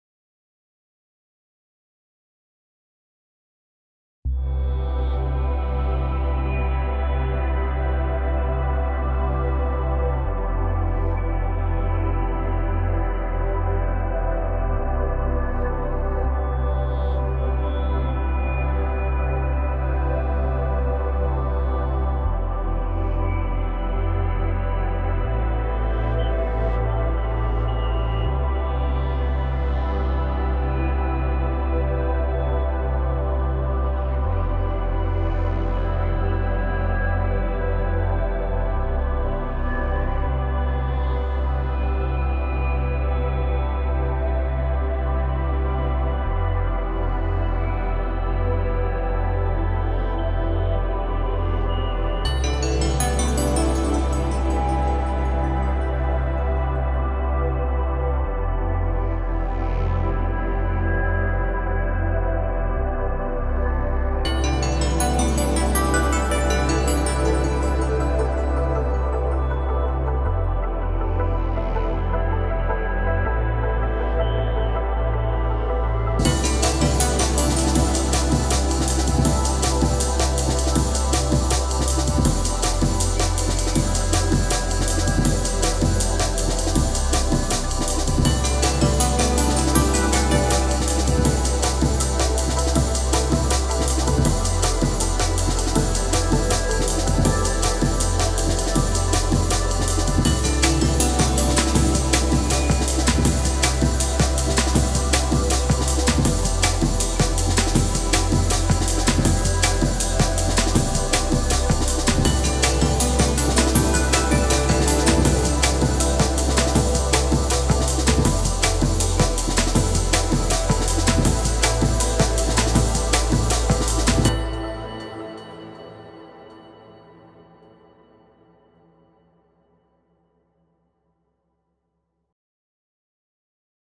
Good Looking Recs Production Sketch/Noodle
Having a bash at producing the more Atmospheric Jungle/DnB of the mid 90's! Enjoy!